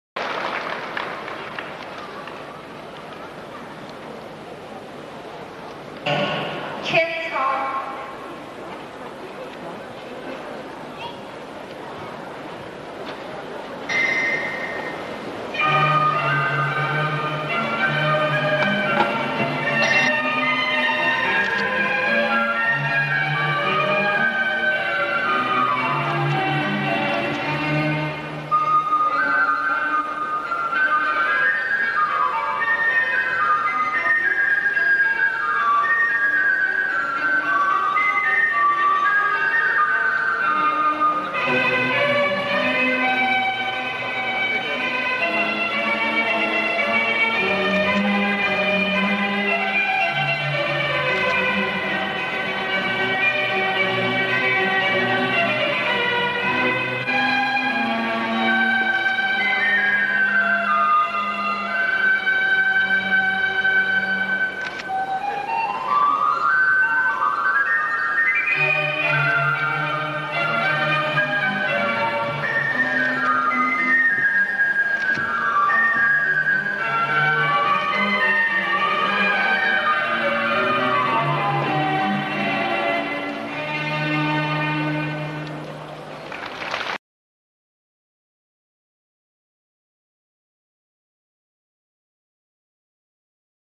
Chinese National team training rhythmic gymnastics Hoop 1992年中国艺术体操国家队训练圈操
我的妻子在1992年在中国国家队的训练录像
Video of my wife training with the Chinese national team in 1992